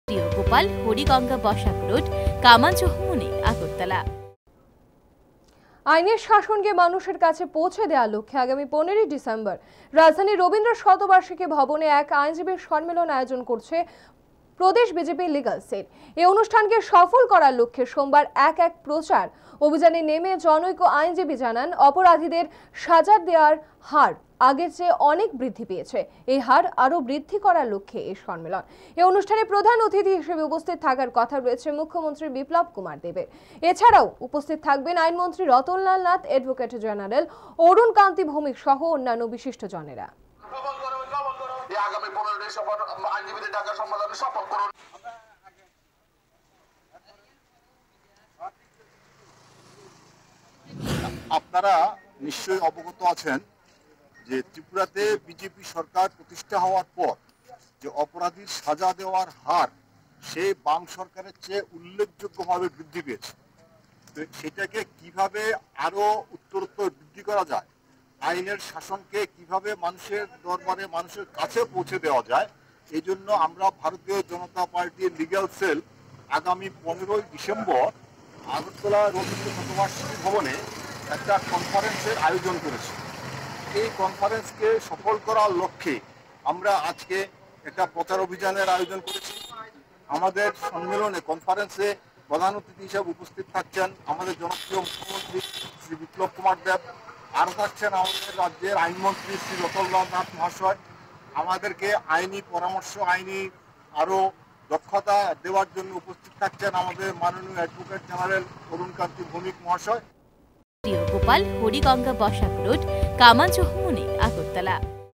আগামী ১৫ ডিসেম্বর রাজধানীর রবীন্দ্র শতবার্ষিকী ভবনে এক আইনজীবী সম্মেলনের আয়োজন করেছে
আইনের শাসনকে মানুষের কাছে পৌঁছে দেওয়ার লক্ষ্যে আগামী ১৫ ডিসেম্বর রাজধানীর রবীন্দ্র শতবার্ষিকী ভবনে এক আইনজীবী সম্মেলনের আয়োজন করেছে প্রদেশ বিজেপি লিগ্যাল সেল।